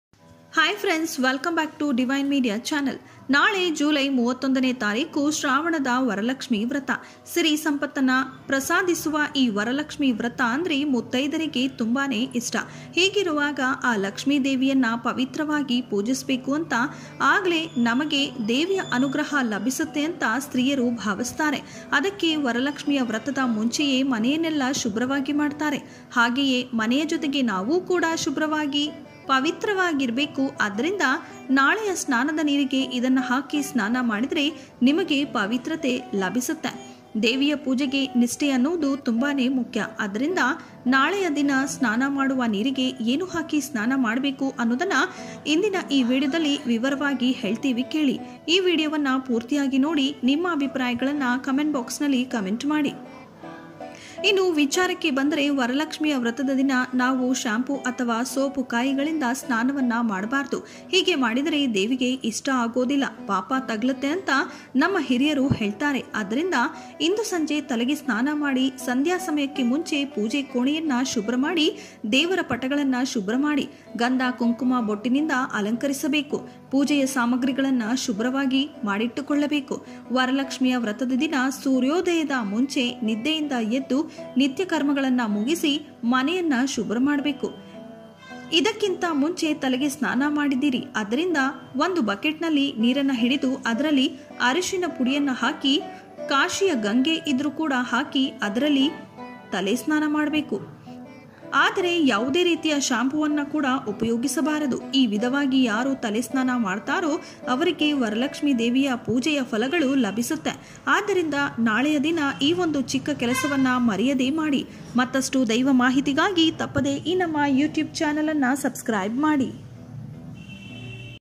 ನಾಳೆ ಸ್ನಾನ ಮಾಡುವ ನೀರಿಗೆ ಇದನ್ನು ಬೆರೆಸಿ ಸ್ನಾನ ಮಾಡಿದರೆ ಅಖಂಡ ಪುಣ್ಯ ಫಲ || ಸಾಕ್ಷಾತ್ ಮಹಾಲಕ್ಷ್ಮಿ ಅಮ್ಮನ ಕೃಪೆ
Divine media Astrology